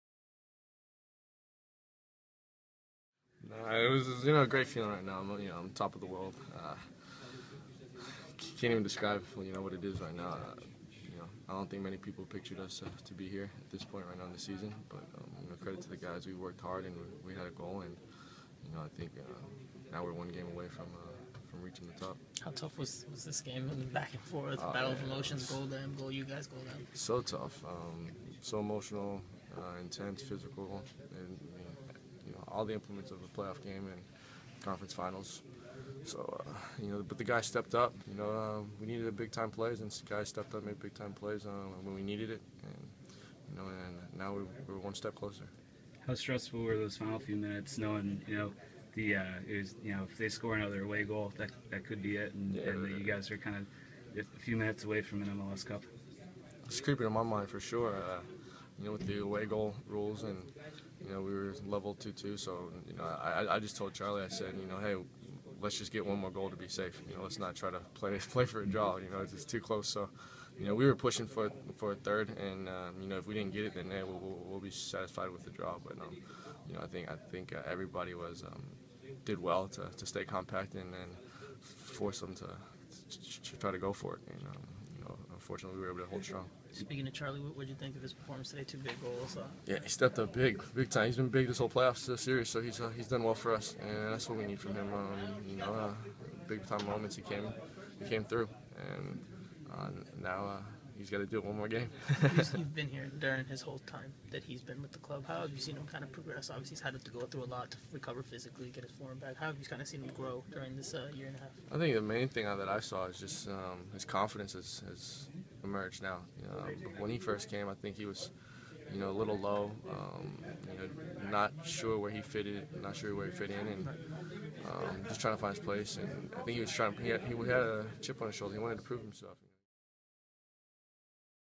Lee Nguyen postgame interview - Revs beat Red Bulls in Eastern Conference final (11/29/2014)
An interview with Lee Nguyen after the New England Revolution defeated the New York Red Bulls 4:3 on aggregate in the 2014 Eastern Conference final.